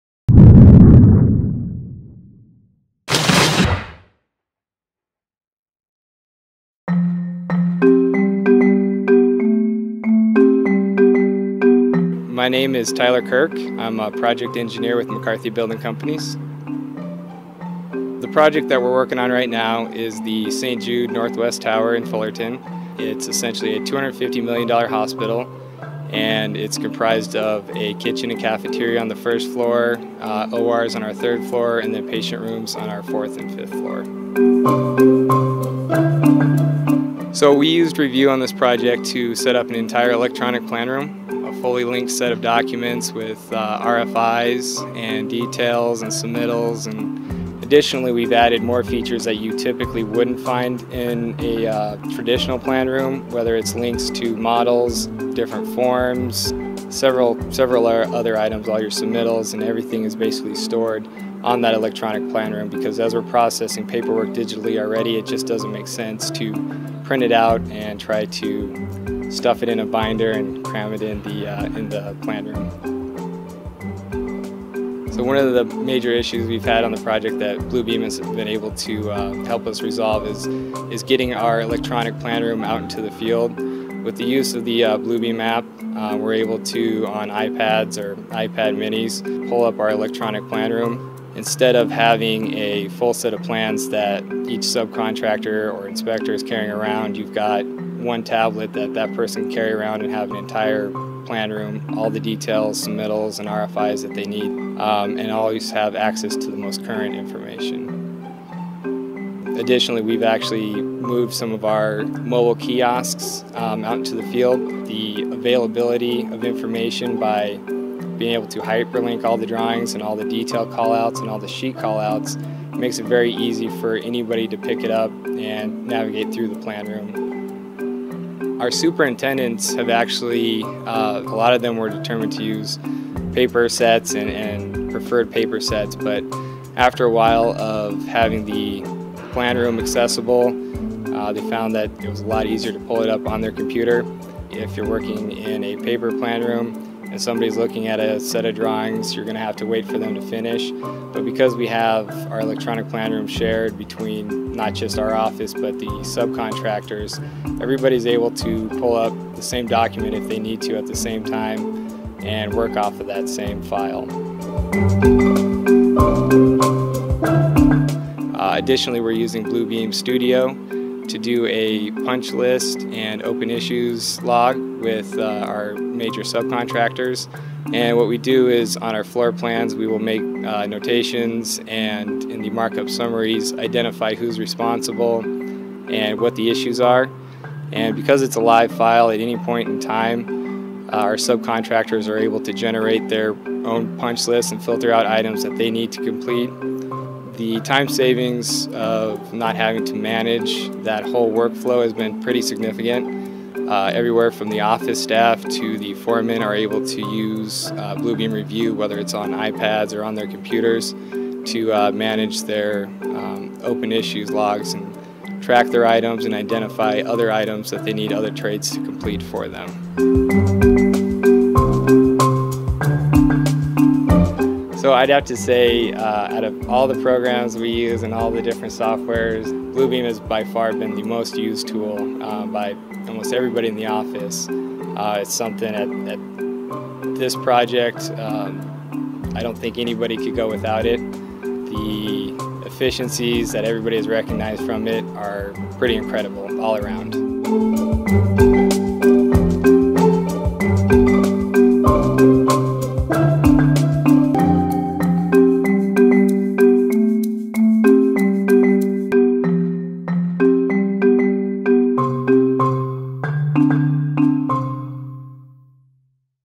McCarthy Uses Bluebeam Revu & Bluebeam Studio on the St. Jude Medical Center Project
McCarthy Building Companies has used Bluebeam Revu to create a digital plan room for the St Jude Northwest Tower proejct in Fullerton, CA. Find out why the Project Engineer says Revu is the most-used software solution on the project.

View, edit, and markup PDFs quickly and easily.